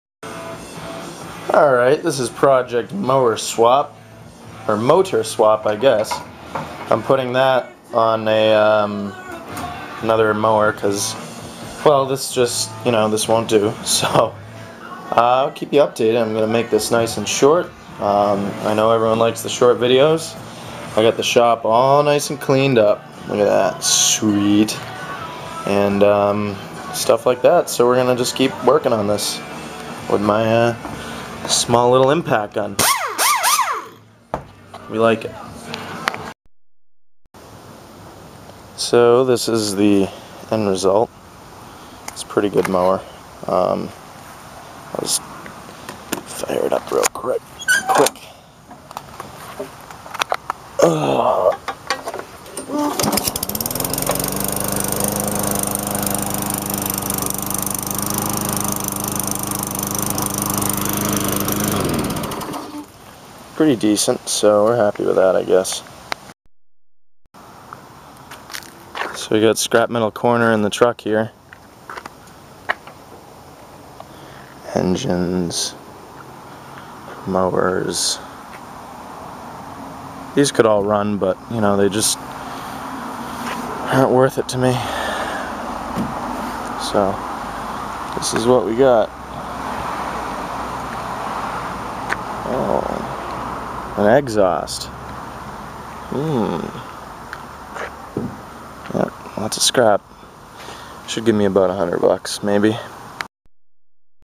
Engine swap on a mower and scrap metal!
i love scrapping